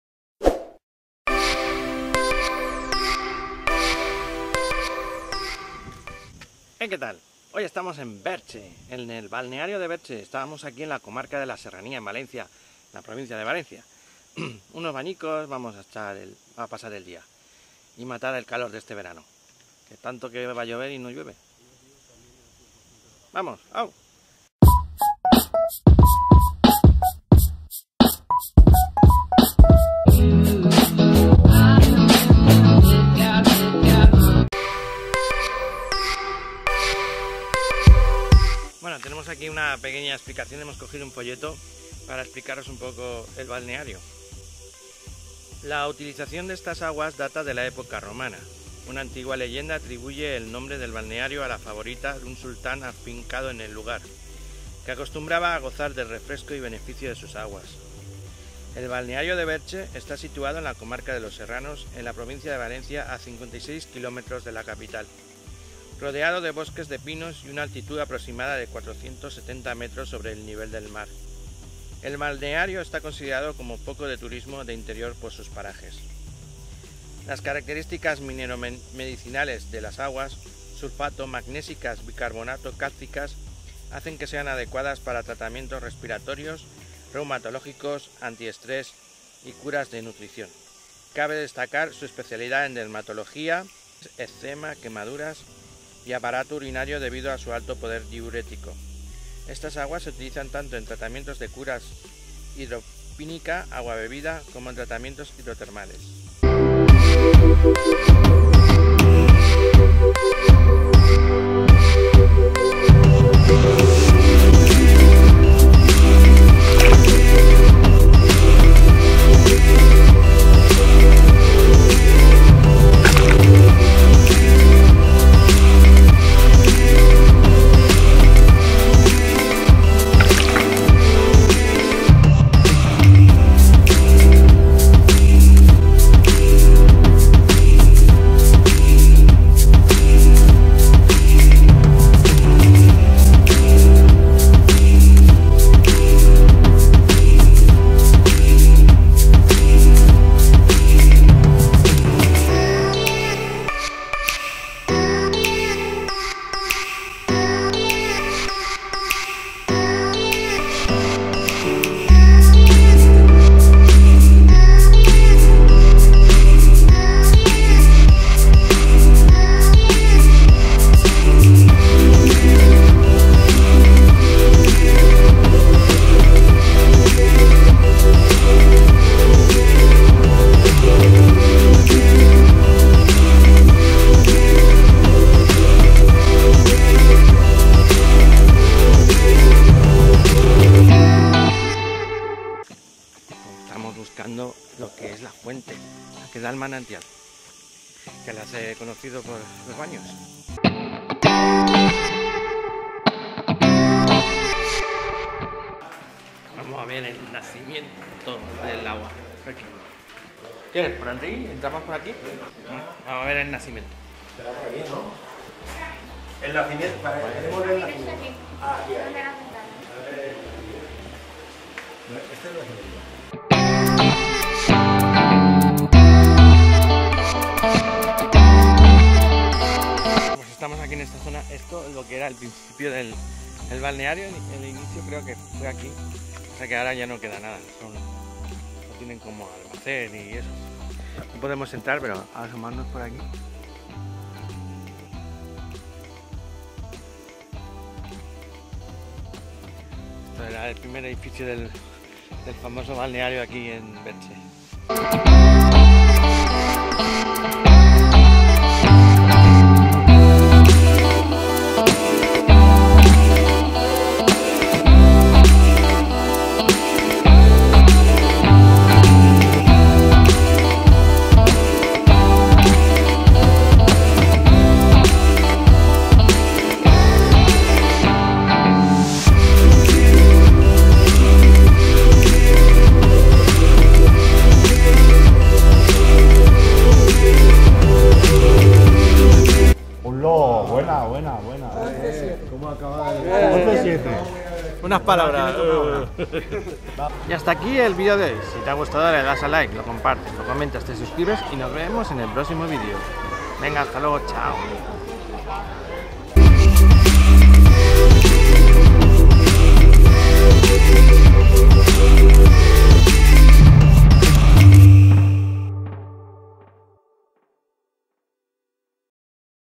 CHAPUZONES y BADMINDTON en VERCHE
JOVI CASINO VLOG 178 : Estuvimos en el Balneario de Verche.

Canal dedicado a casi todo que me rodea, aficiones, gustos, trayectoria como actor, Cultura, viajes..